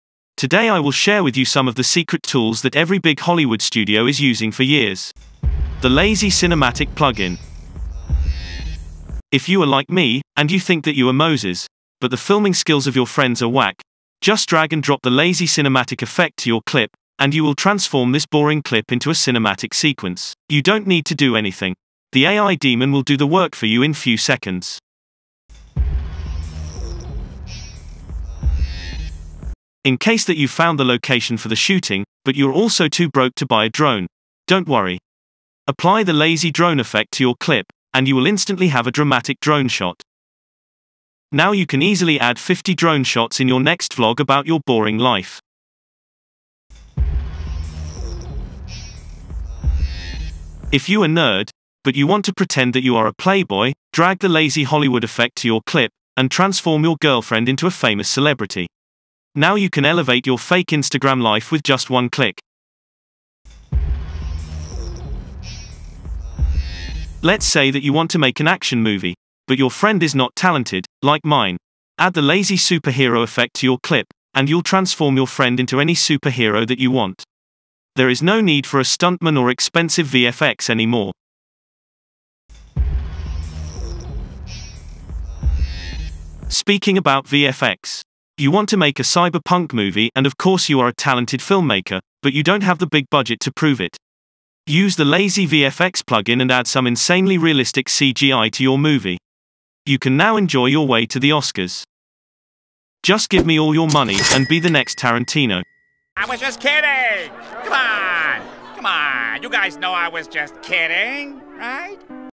5 Hollywood Secret Tools | DaVinci Resolve 17
Here are 5 of the Hollywood secret tools
The ultimate cinematic plugin
BUY it for just 999.999 $/Day

Well, it's April fools day... also I was to lazy to make a tutorial.

you can send them here :

BTC : 1Ecg6F8UnFHnVxsKhbkecqtge11vBhqS5
ETH : 0xB643C1C8B6C75bcB3426F40f8514CB89Fc294abC